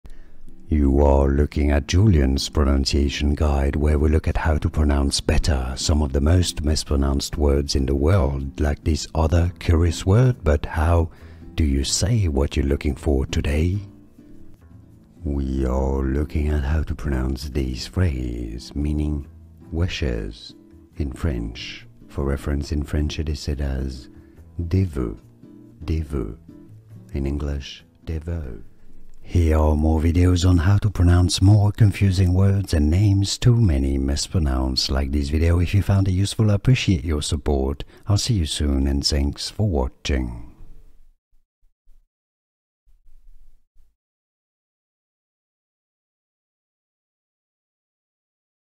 How to Pronounce Des Voeux (correctly!)
Welcome! Here you will hear How to Pronounce Des Voeux (correctly!) i.e. the “correct” pronunciation.

You can skip the intro through the time stamps below:
00:00 - Pronunciation Intro
00:15 - How to Pronounce Des Voeux (correctly!)